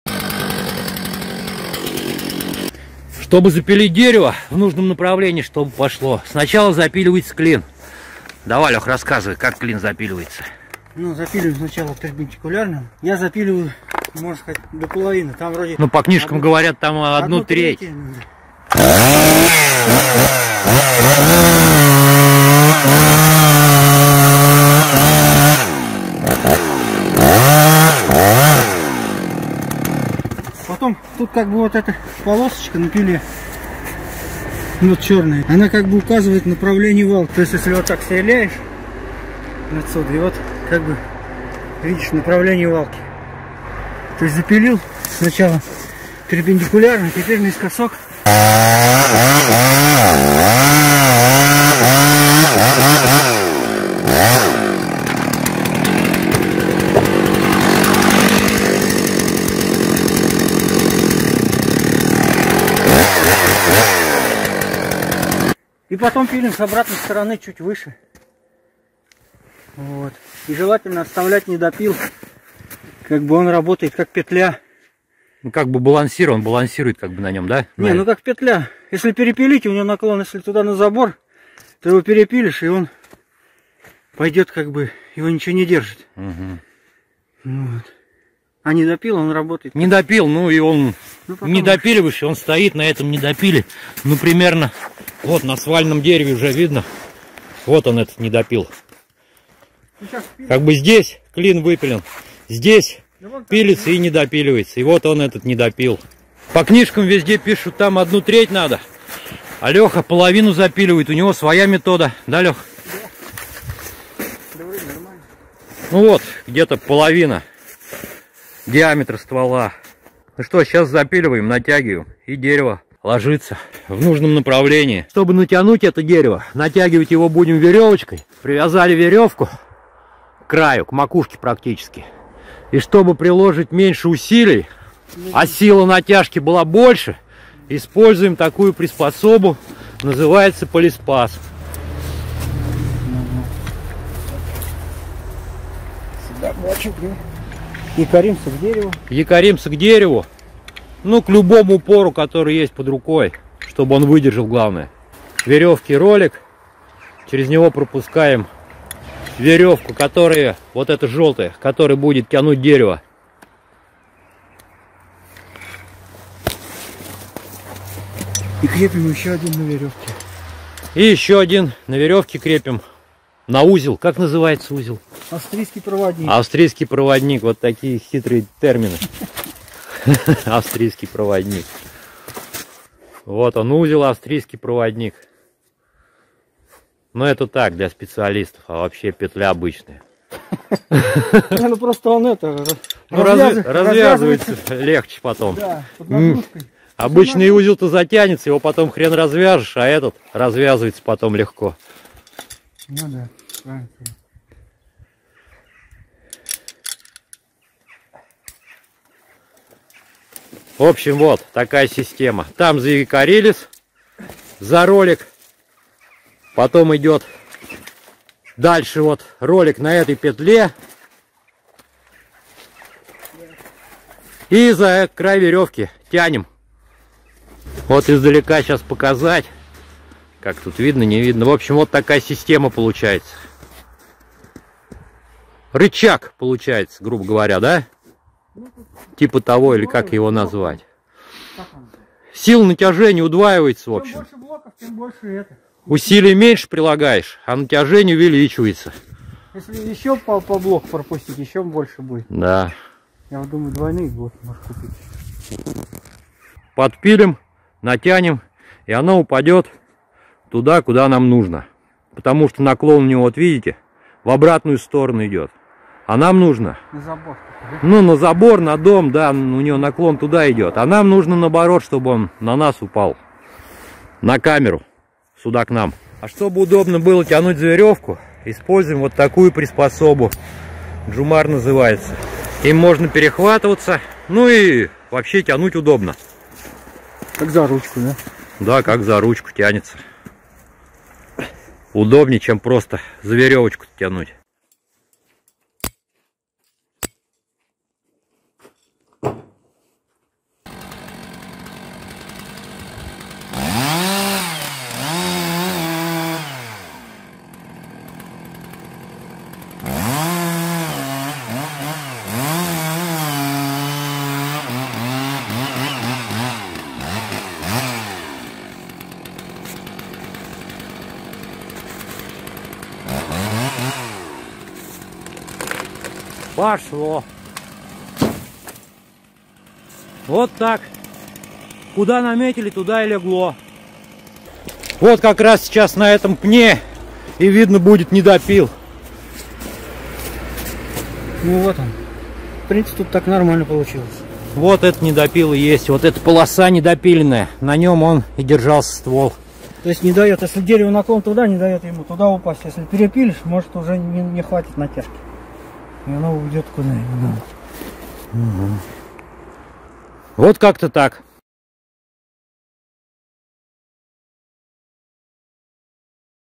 Как спилить дерево чтобы оно упало в нужном направлении
Как правильно спилить дерево бензопилой в нужном направлении